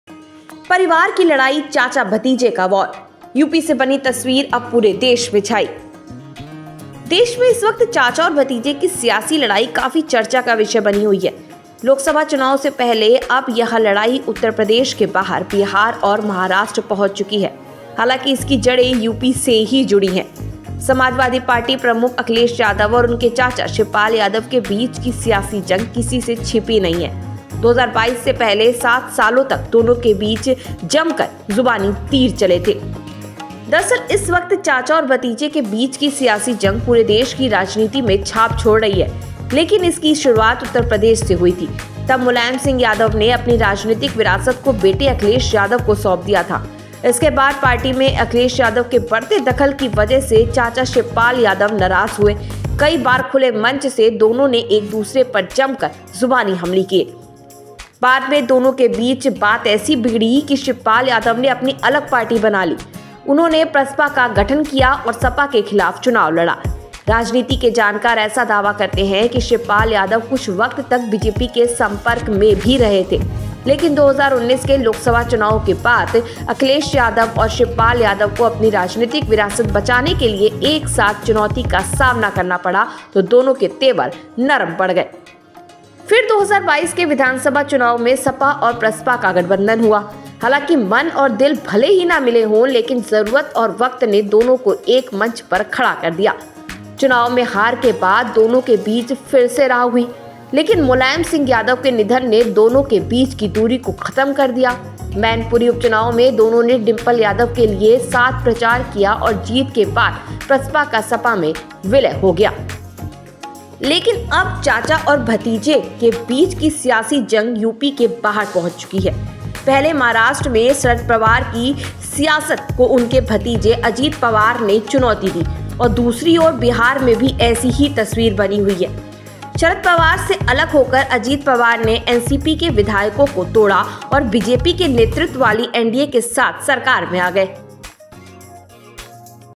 परिवार की लड़ाई, चाचा-भतीजे का वॉर | यूपी से बनी तस्वीर, अब पूरे देश में छाई
देश में इस वक्त चाचा और भतीजे की सियासी लड़ाई काफी चर्चा का विषय बनी हुई है. लोकसभा चुनाव से पहले अब यह लड़ाई उत्तर प्रदेश के बाहर बिहार और महाराष्ट्र पहुंच चुकी है. हालांकि इस जड़ें यूपी से ही जुड़ी हैं. समाजवादी पार्टी प्रमुख अखिलेश यादव और उनके चाचा शिवपाल यादव के बीच की सियासी जंग किसी से छीपी नहीं है. 2022 से पहले सात सालों तक दोनों के बीच जमकर जुबानी तीर चले थे.